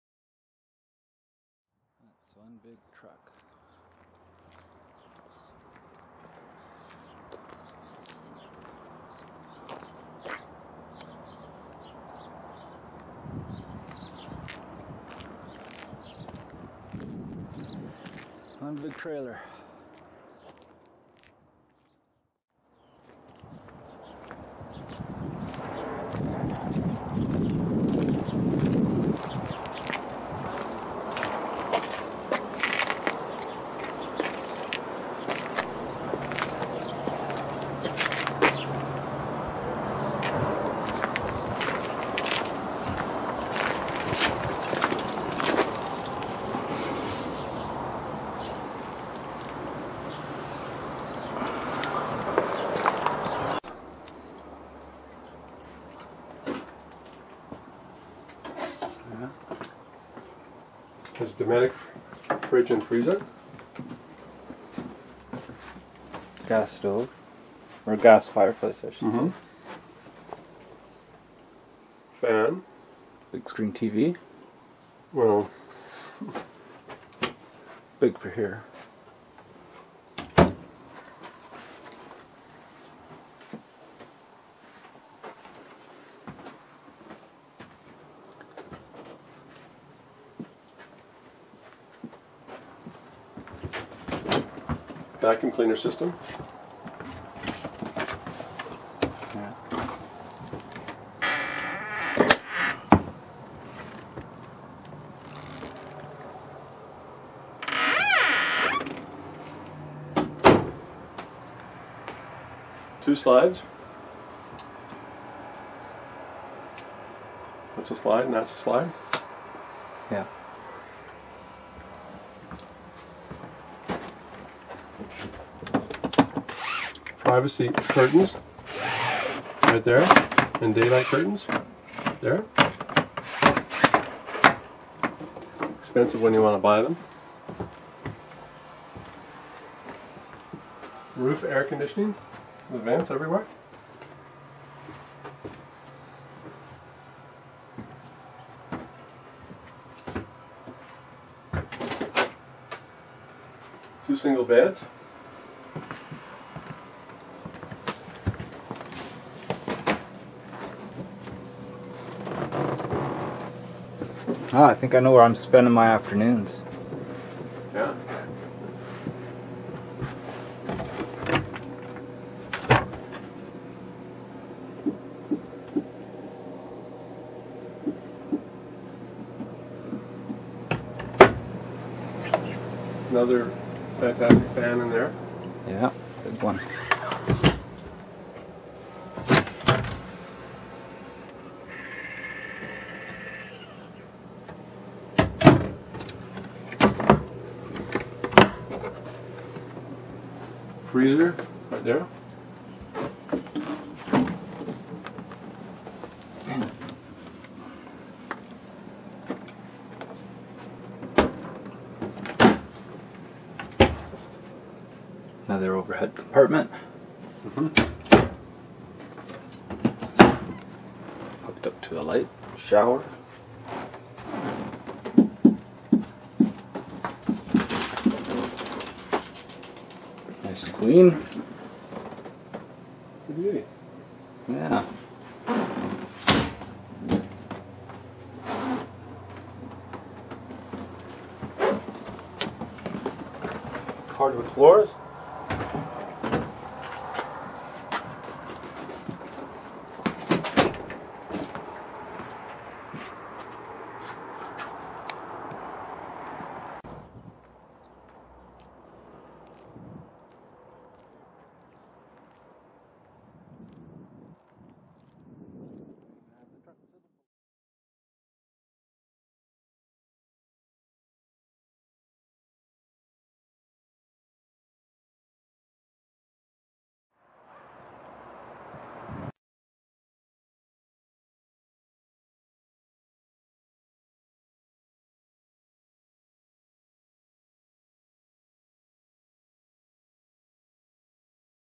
13' Titanium Trailer For Sale
Join Don on a tour of our used 31' Titanuim Trailer for sale. Many options included, with 2 slide-outs, fridge, stove, freezer, master bedroom with 2 single beds, security, shower, A/C and more...